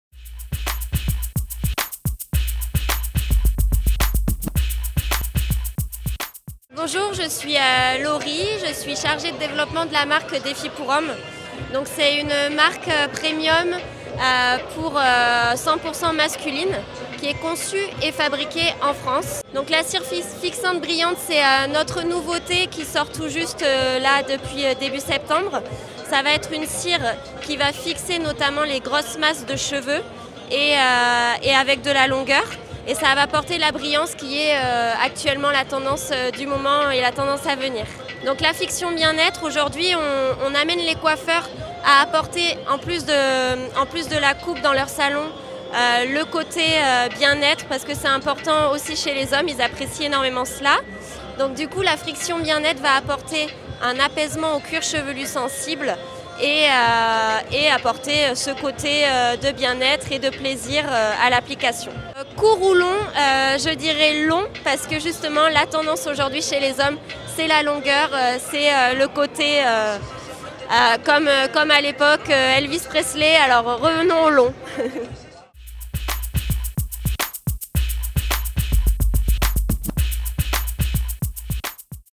Reportage MCB by BS 2019 - DÉFI pour Homme
Nouveau chez DÉFI pour Homme : la cire fixante + friction bien-être, présenté par Laurie Nawrot, chargée de développement.